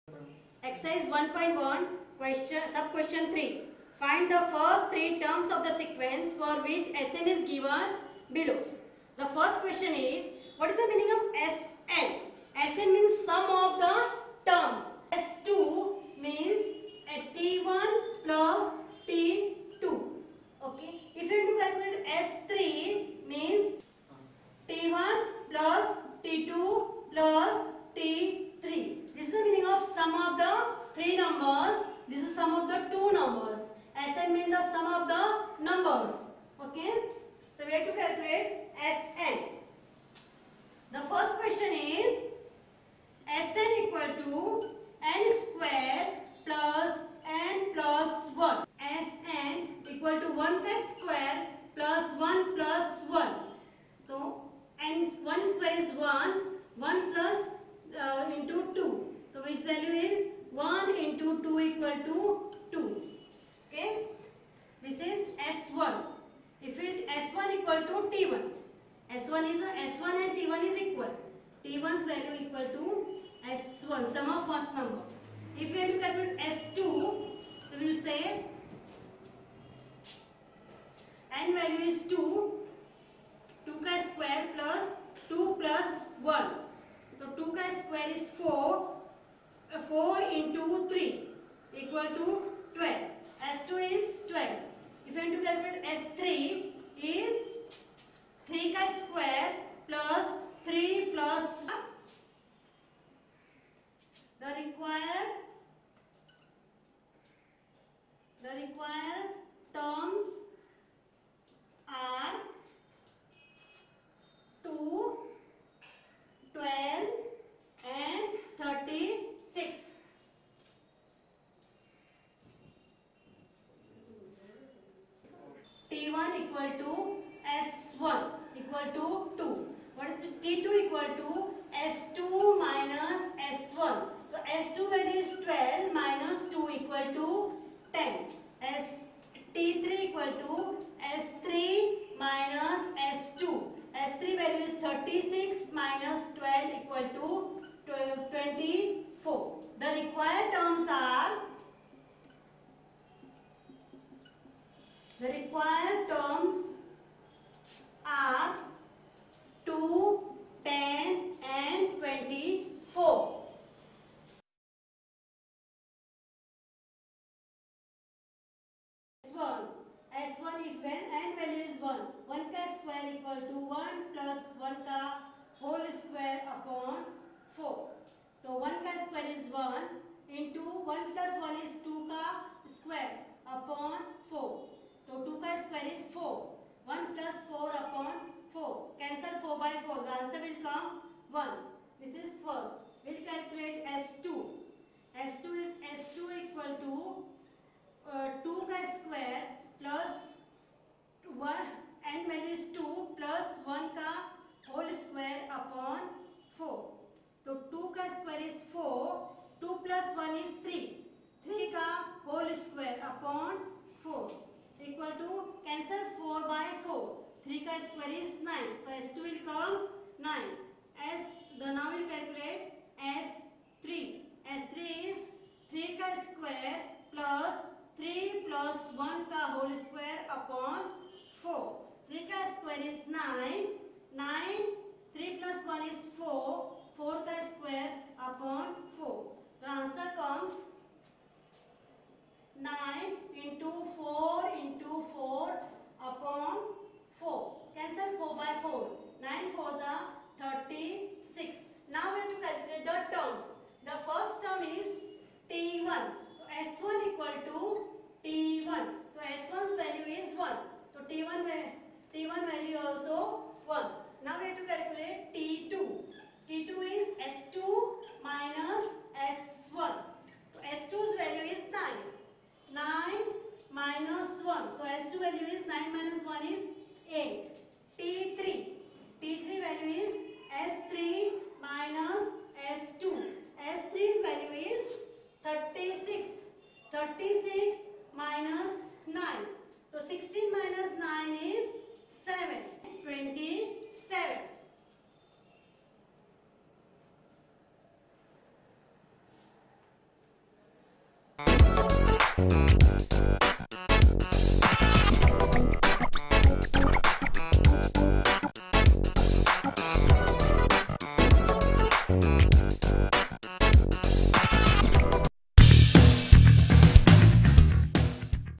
SSC 10th std Maths-Arithmetic and Geometric Progression Exercise Algebra Chapter 1 .1.3
Arithmetic Progression and Geometric Progression Exercise Algebra Chapter 1 .1.3 Ssc
(tenth standard)

This sum is based on 10th standard's  'Algebra ' and 'Maths' subject.  Maharashtra board textbook  Chapter 1 sum of Arithmetic progression and geometric progression. Updated by latest syllabus